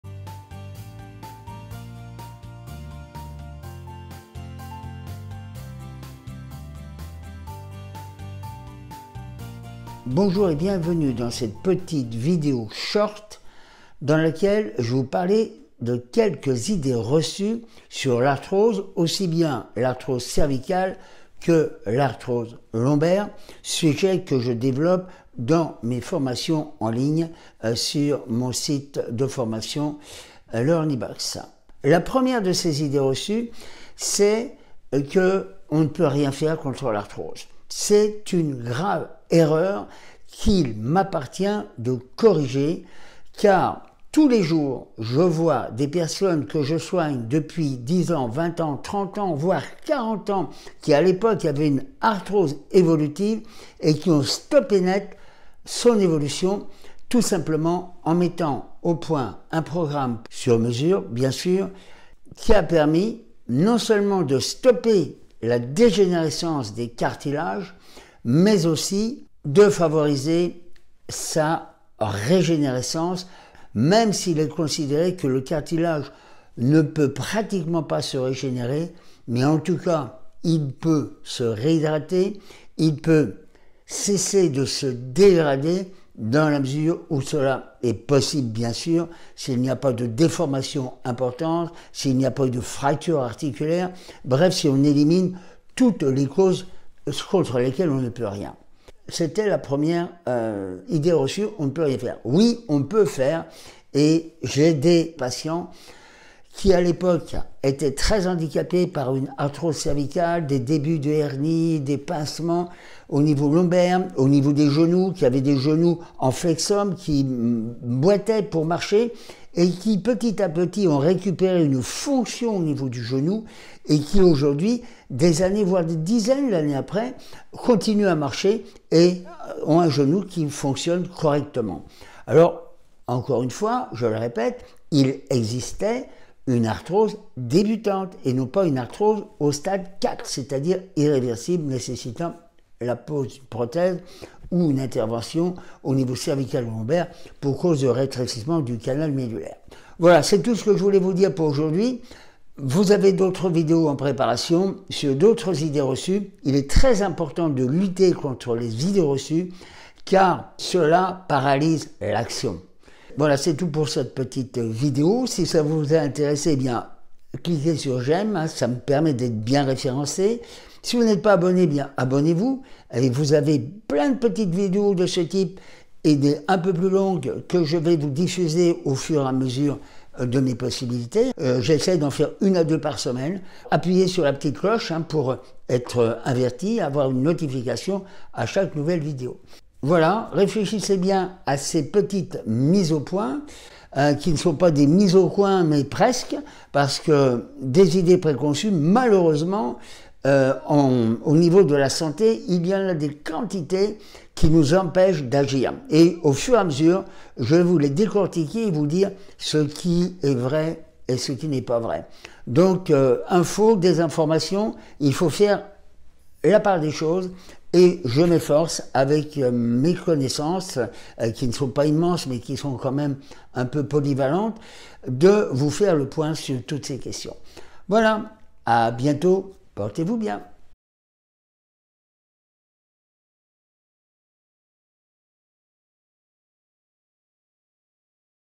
Vidéo Arthrose : 1ère idée fausse par Guy Roulier
Les idées fausses paralysent l'action et empêchent la mise en place d'une prévention santé durable dans la plupart des maladies dégénératives dont l'arthrose fait partie.
Les débusquer libère l'esprit et remotivent celles et ceux qui pensaient leur cas désespéré. Le corps humain possède des capacités de régénération incroyable à la condition d'utiliser les bonnes synergies, celles qui corrigent les causes réversibles.
Apprendre à prendre sa santé en main est le premier pas à franchir pour mettre en place un programme d'entretien efficace.